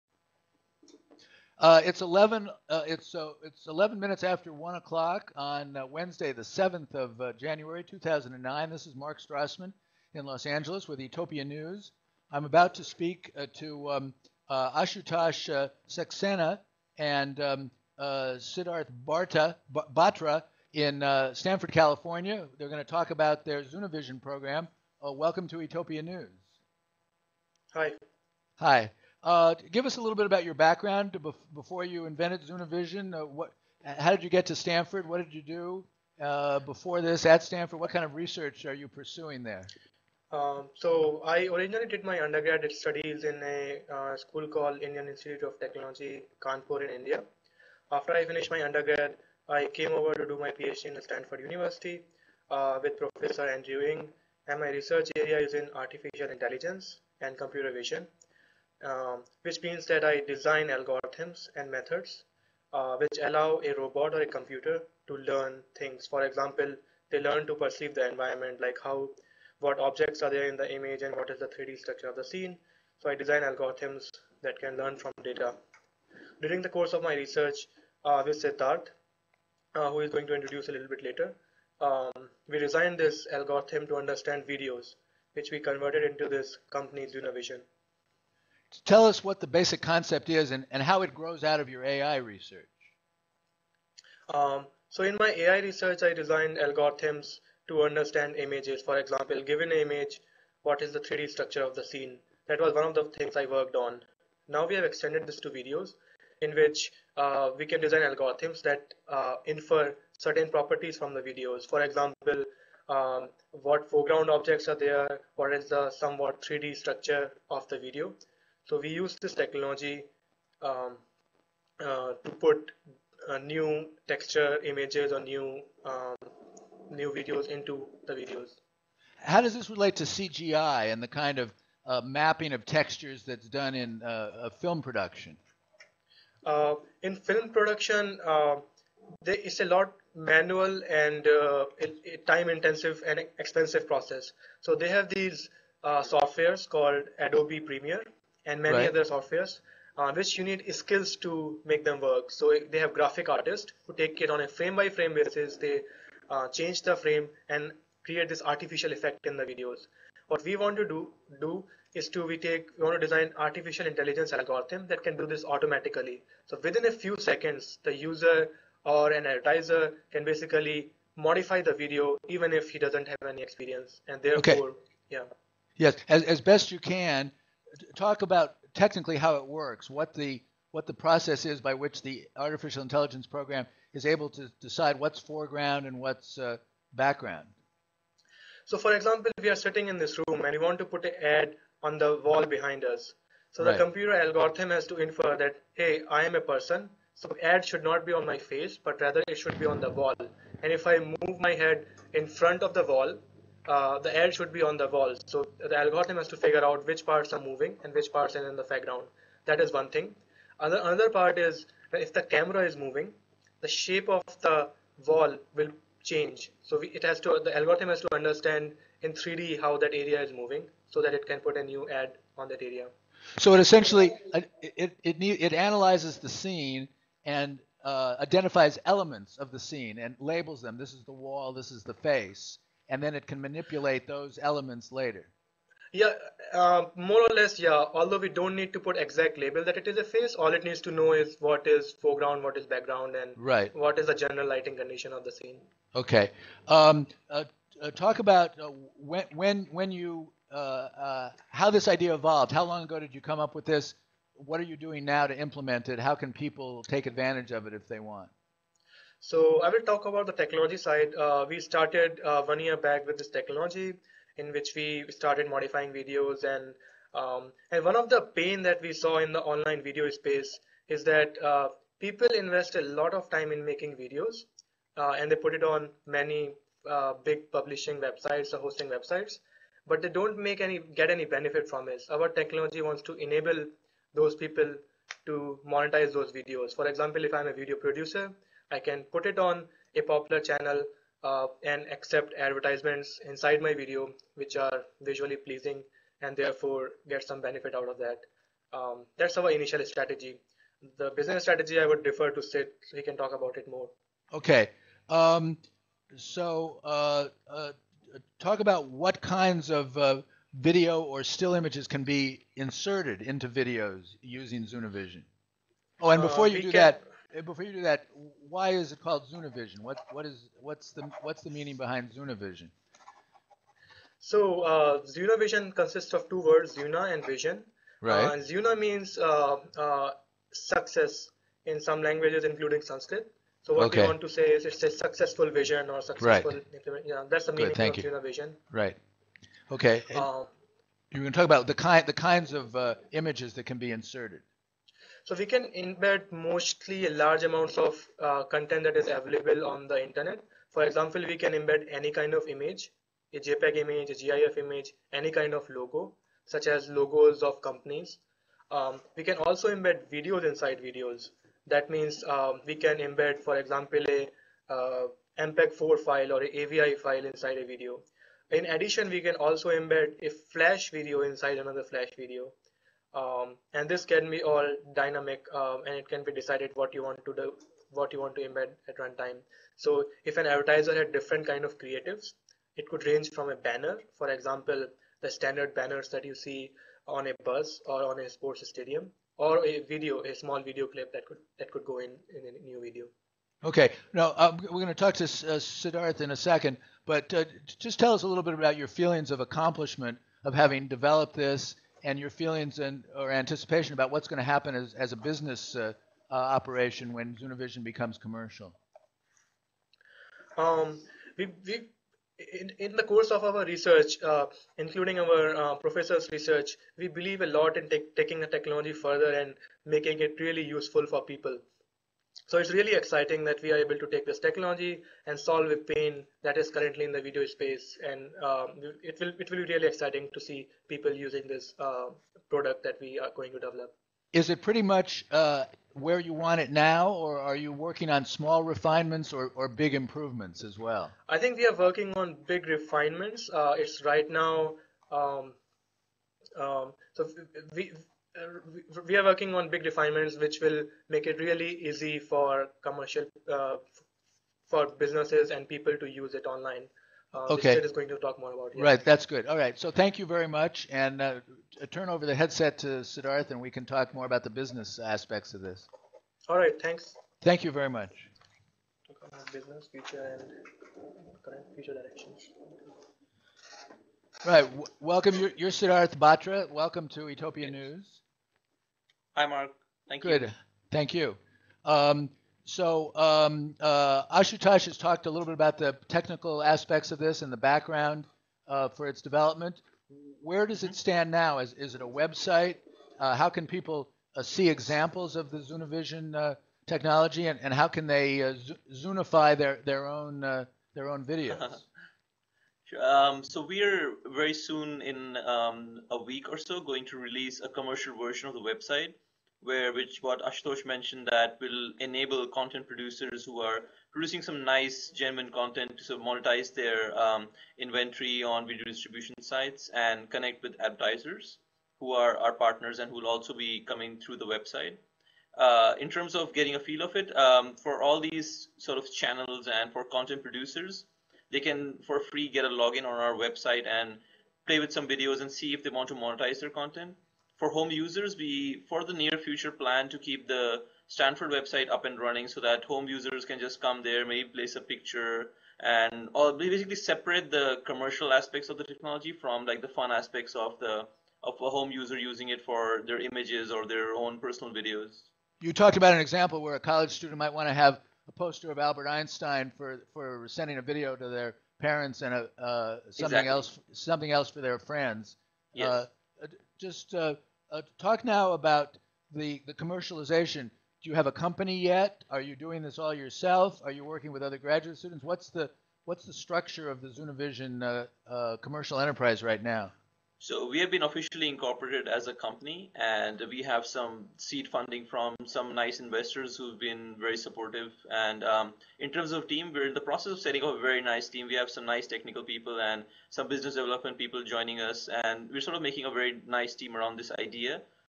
exclusive interview with Ashutosh Saxena and Siddarth Batra, co-founders, ZunaVision, part 1
Ashutosh Saxena & Siddarth Batra, computer science graduate students at Stanford, talk about ZunaVision, an algorithm that embeds images and videos in videos, recorded remotely from Stanford University on January 7, 2009